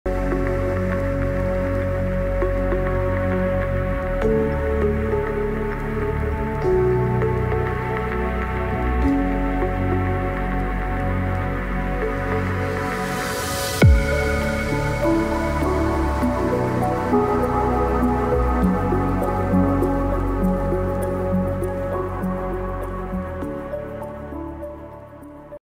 Waterfall Jungle rope walk
Waterfall rope walk in dense jungle of Costa Rica